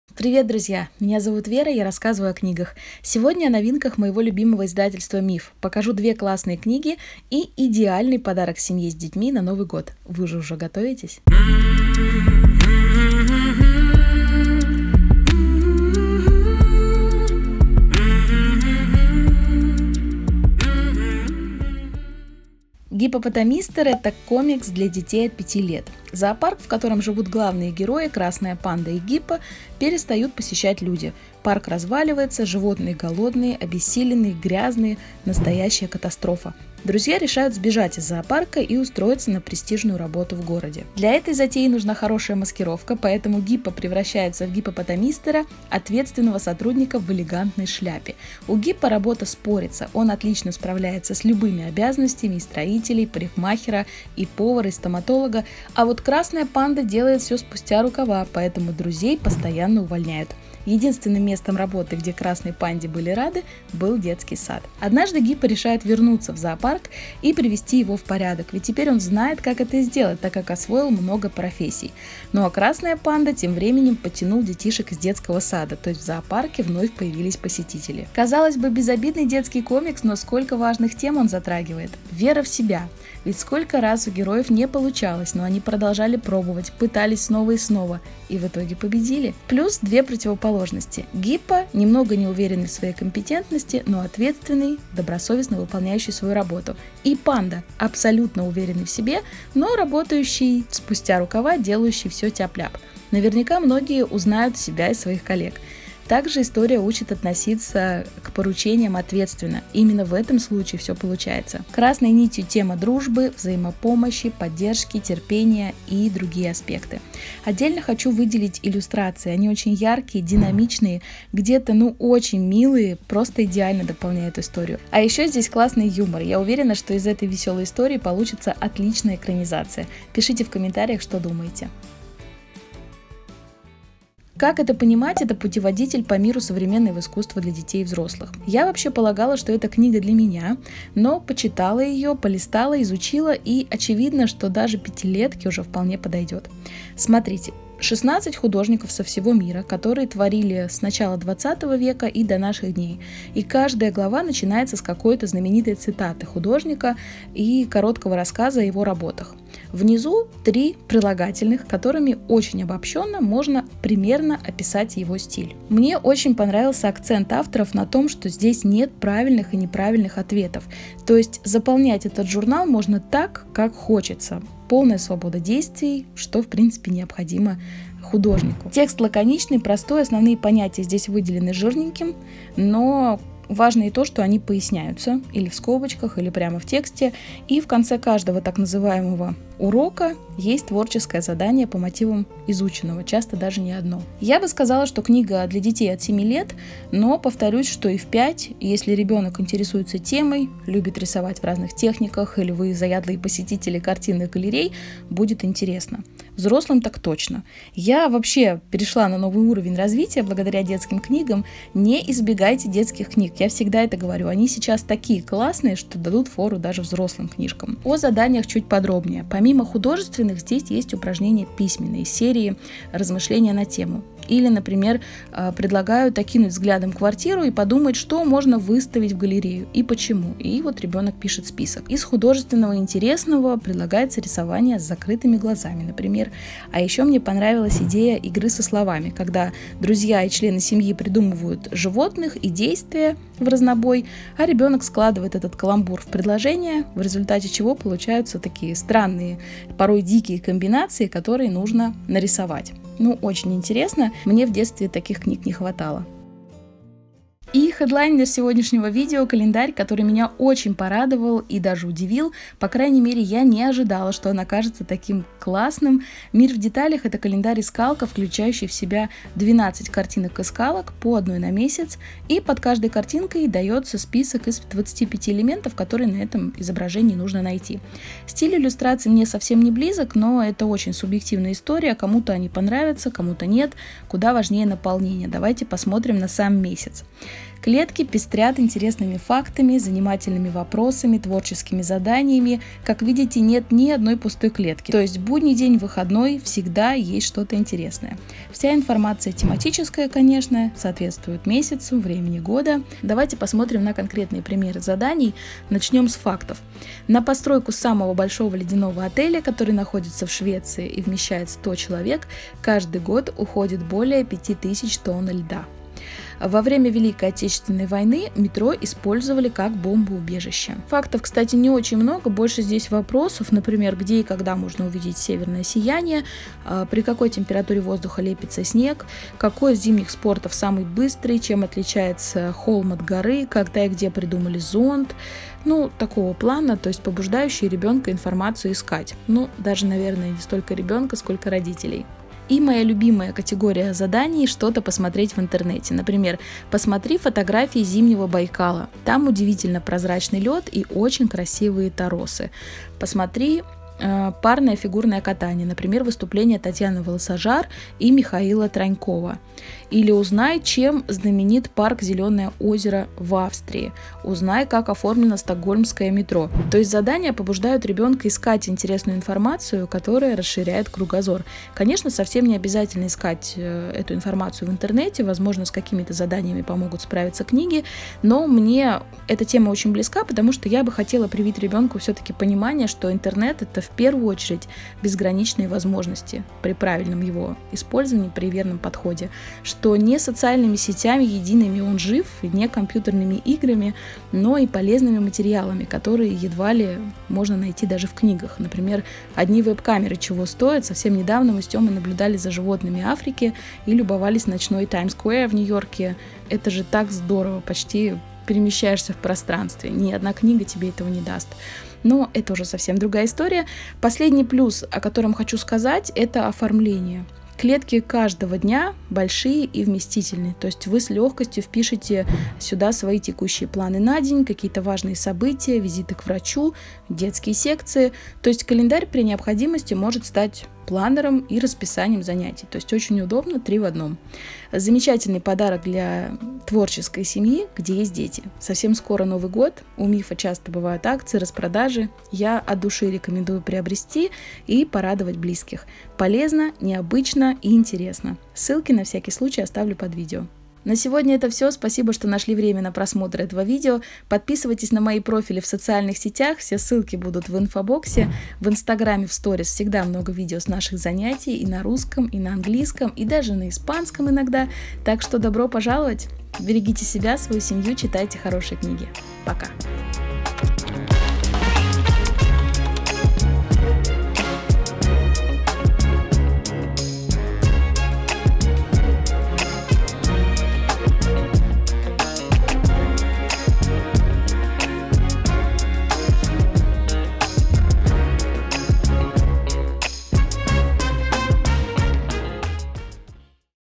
КАЛЕНДАРЬ-ИСКАЛКА С ЗАДАНИЯМИ / ГИППОПОТАМИСТЕР / КАК ЭТО ПОНИМАТЬ/ VERA PEK
Новинки от издательства МИФ. К сожалению, вторая часть заказа не успела прийти к съёмкам, поэтому совсем скоро покажу ещё одну порцию новых книг МИФа. Сегодня рассказываю о весёлом и не пустом комиксе для детей от 5 лет «Гиппопотамистер», гиде по современному искусству для детей и взрослых «Как это понять?!» и показываю замечательный календарь-искалку «Мир в деталях». Последний меня просто очаровал! Жду ваши комментарии. И спасибо, что вы со мной. :)
Книги из видео:
Гиппопотамистер:
OZON: -